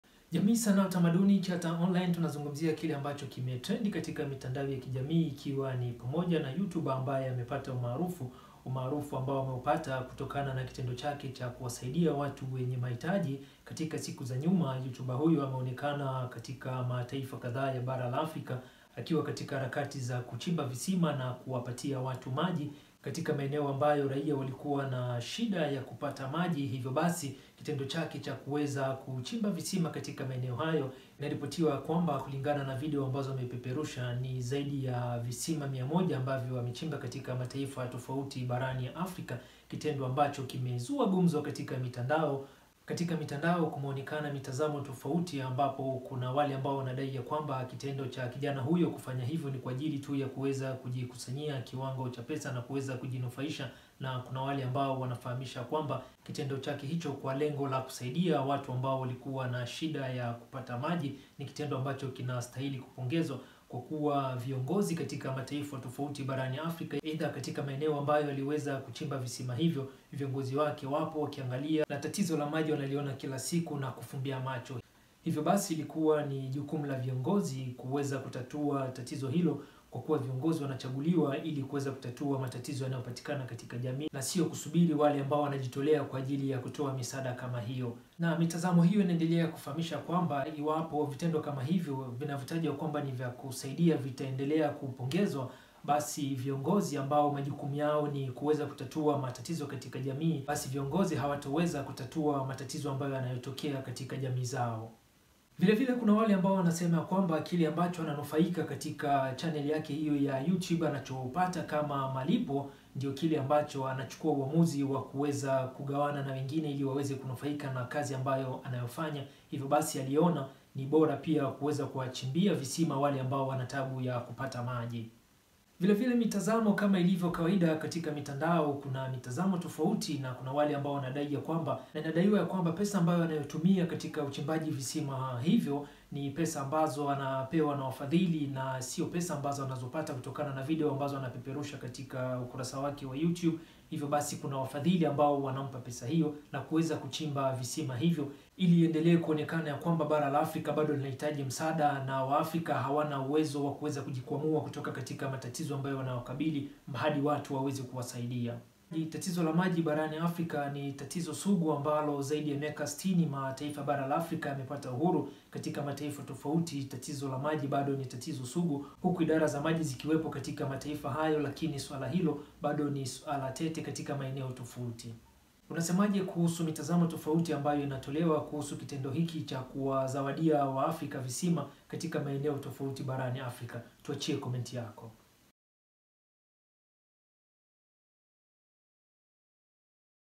Kwanini alaumiwe kwa kuchimba visima Afrika ?
Msaada wa kuchimba visima kwa lengo la kuwapunguzia tabu ya kupata maji safi wakazi katika vijiji tofauti barani barani , msaada uliotolewa umepelekea  mitazamo tofauti na viongozi kutupiwa lawama kwa kushindwa kutekeleza  majukumu yao  katika jamii wanazoongoza ...

Kwa mengi zaidi , jiunge na YouTube Chanel yetu

Production : - Kyata Online @kyataonline